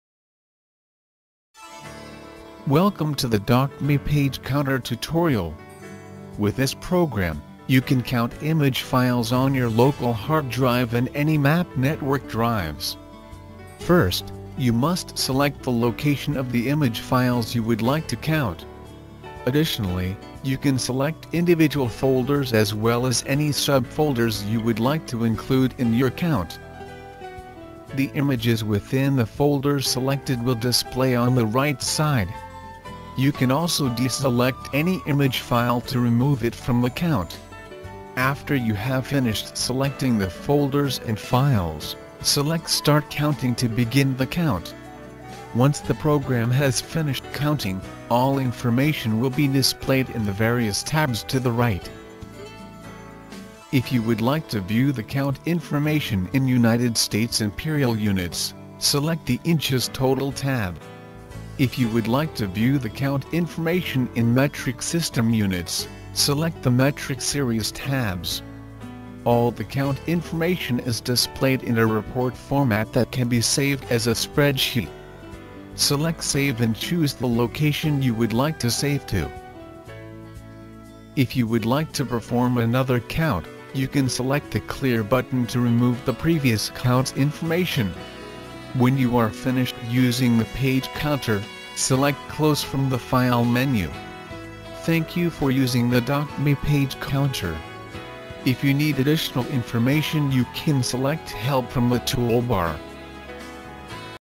Tiff PDF Page Counter
Counts Tiff and PDF files in Directories and sub-directories, Generate a Full report by document size, color scheme, number of documents.  Simple and friendly interface. Export the results to a CSV file, with the detail for each page: its size, color scheme, filename, page number.  Works with English sizes and Metric sizes. Multilingual software: English, French, Spanish, Portuguese.